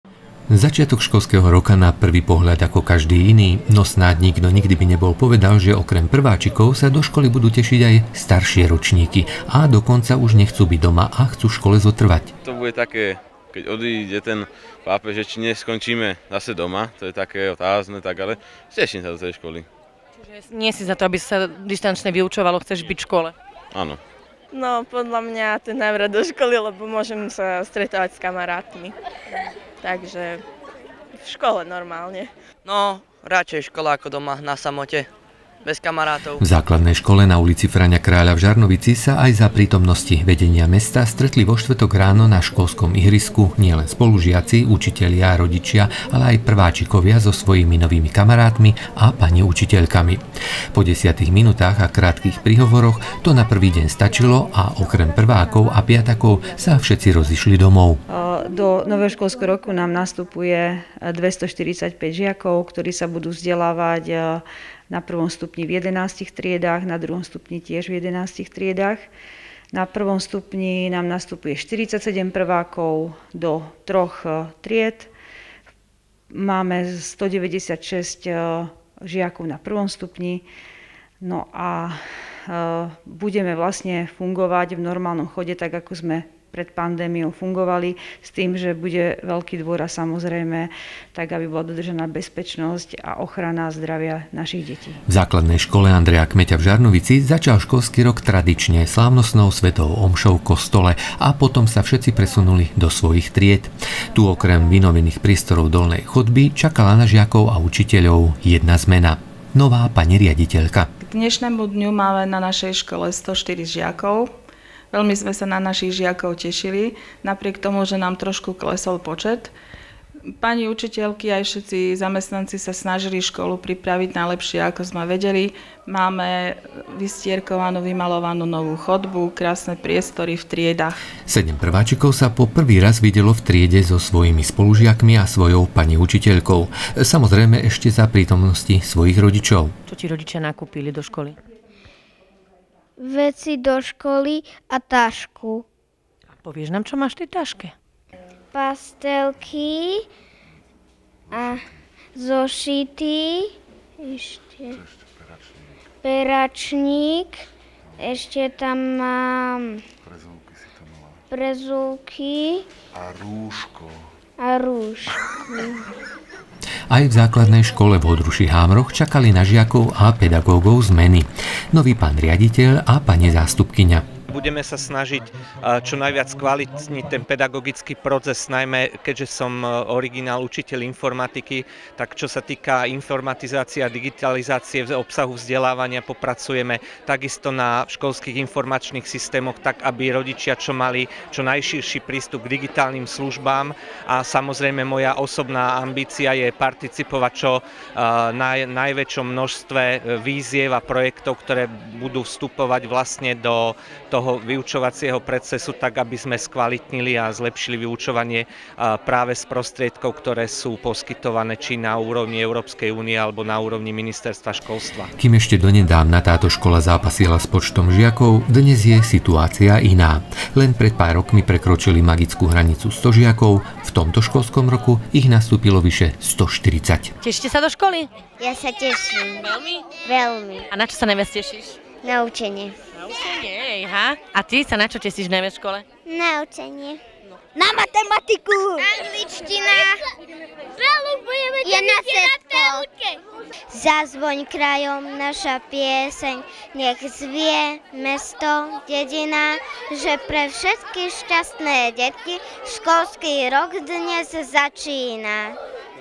Nový školský rok
Reportáž z otvorenia nového školského roka v okrese.